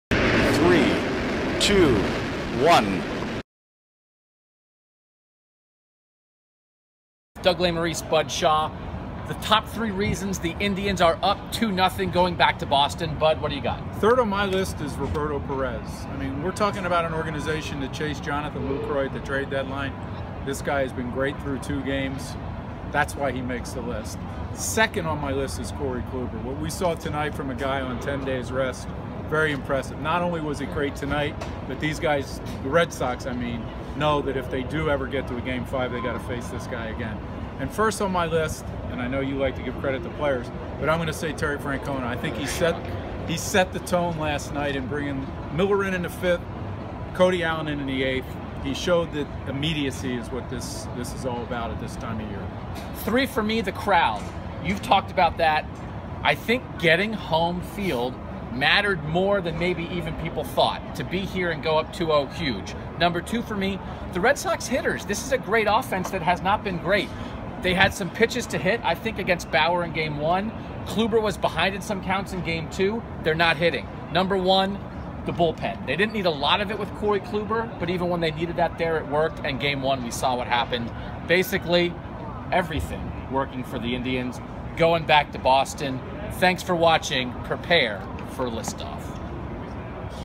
3 reasons the Indians are leading Boston 2-0 in the American League Division Series
Bud Shaw and Doug Lesmerises were at Progressive Field after the Indians win Friday night to give their three reasons why Cleveland leads Boston 2-0 in the American League Division Series.